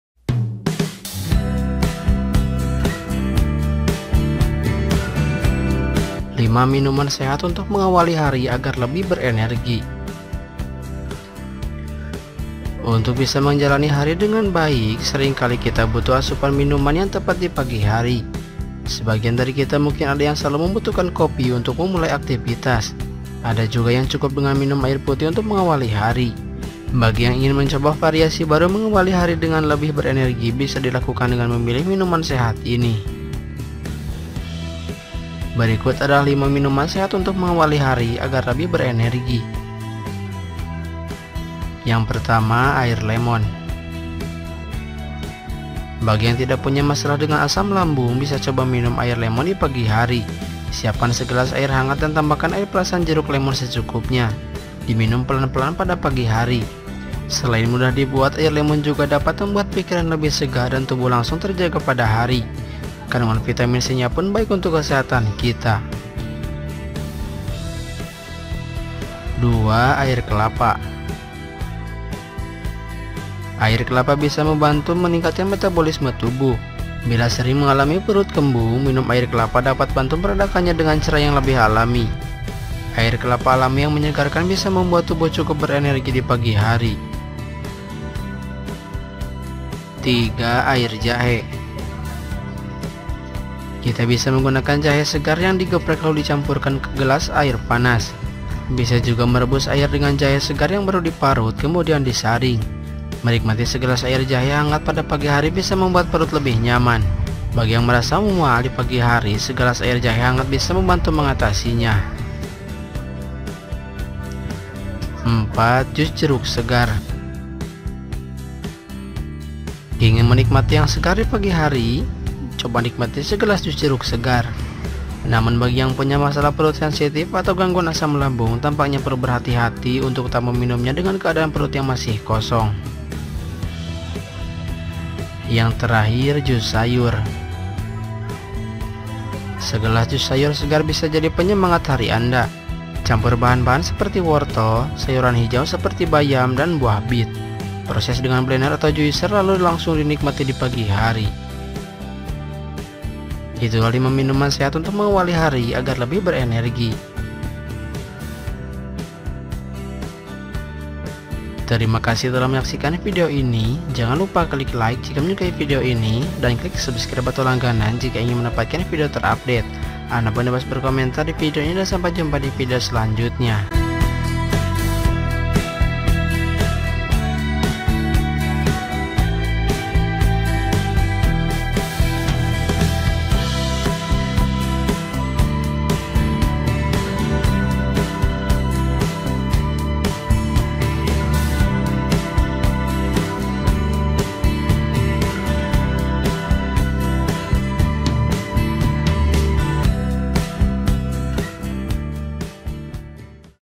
5 Minuman Sehat untuk Mengawali Hari agar Lebih Berenergi
Video kali ini tentang 5 minuman sehat untuk mengawali hari agar lebih berenergi.

Untuk bisa menjalani hari dengan baik, seringkali kita butuh asupan minuman yang tepat di pagi hari. Sebagian dari kita mungkin ada yang selalu membutuhkan kopi untuk mulai beraktivitas. Ada juga yang cukup dengan minum air putih untuk mengawali hari. Bagi yang ingin mencoba variasi baru mengawali hari dengan lebih berenergi bisa dilakukan dengan memilih minuman sehat ini.

Berikut adalah 5 minuman sehat untuk mengawali hari agar lebih berenergi.